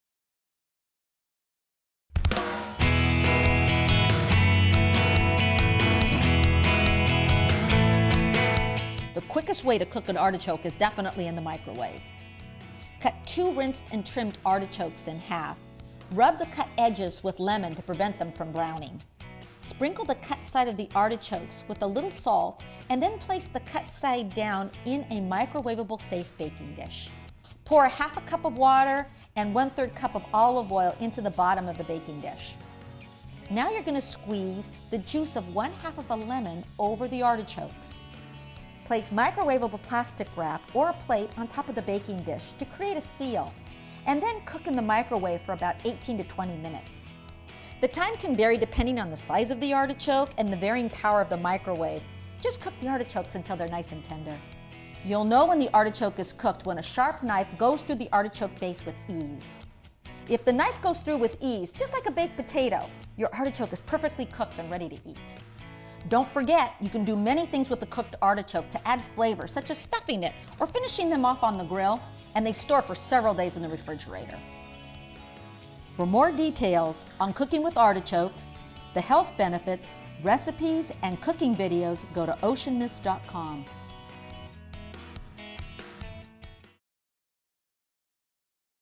How to Mircowave an Artichoke
Find out how easy it is to cook an Artichoke in the microwave with our step-by-step microwaving an Artichoke video. For more Artichoke preparation and cooking videos,